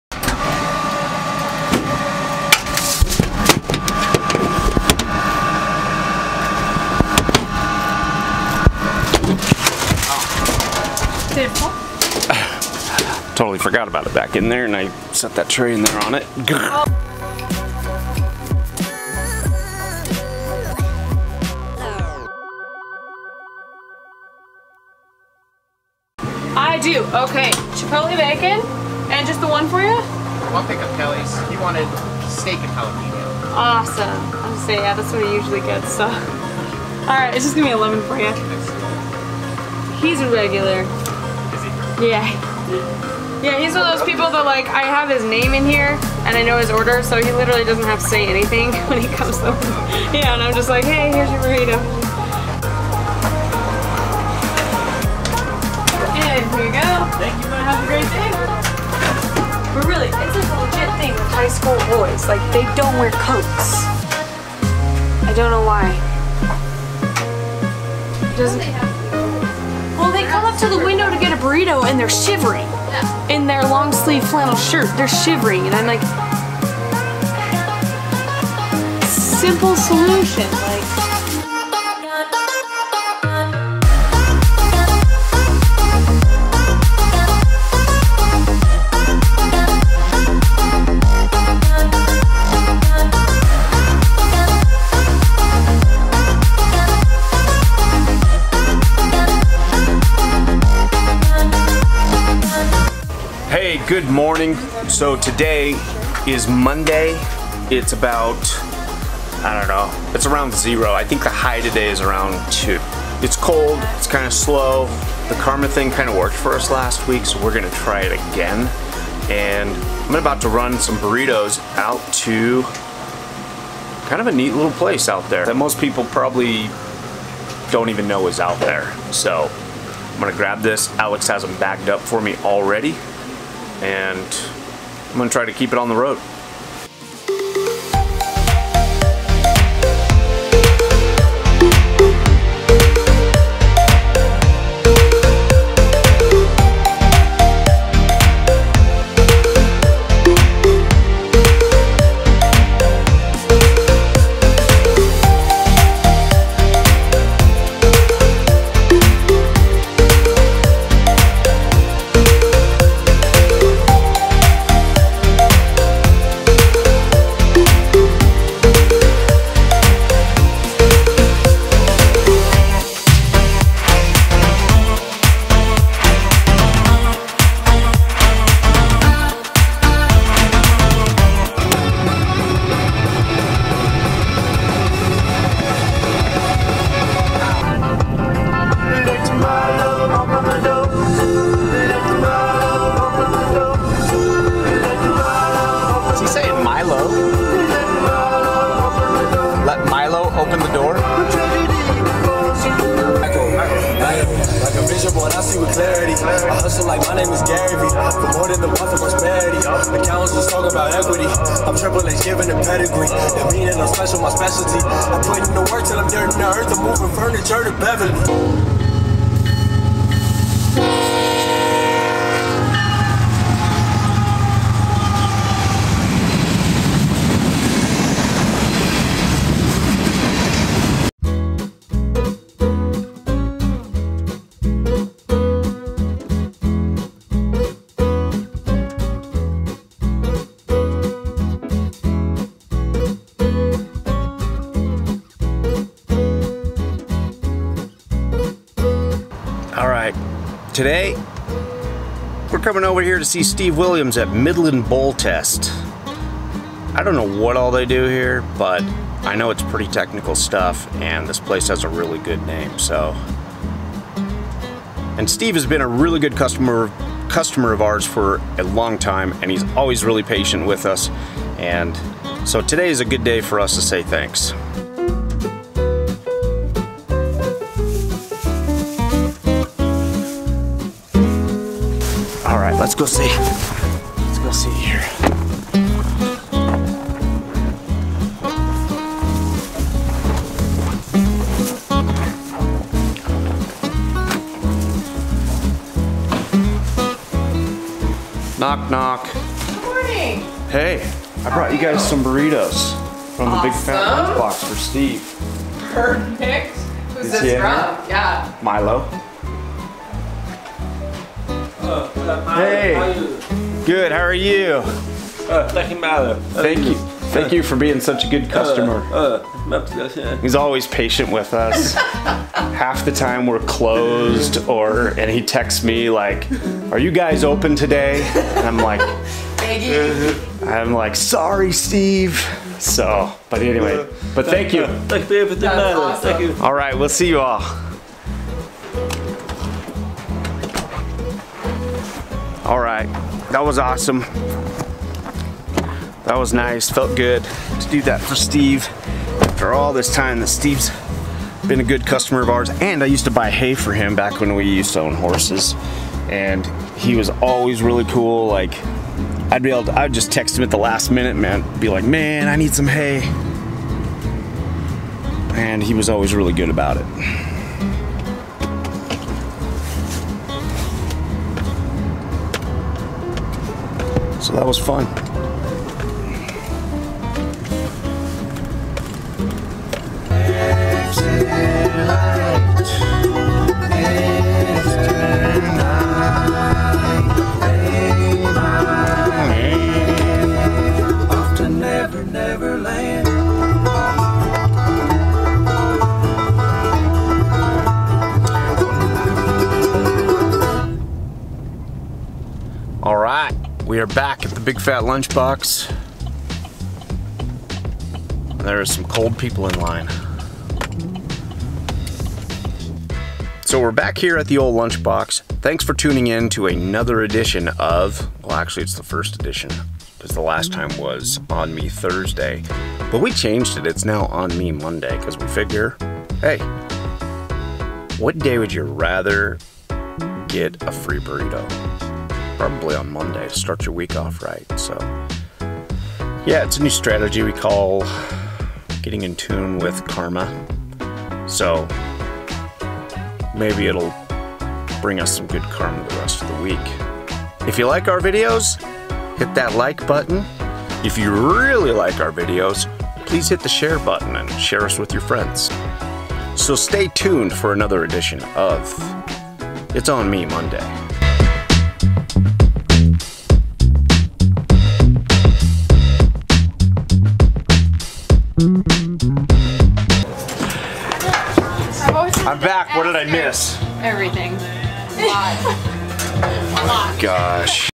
It's "On Me Monday" Ep. 2
Alex rants about high school boys and we surprise a great local business with Free Breakfast Burritos!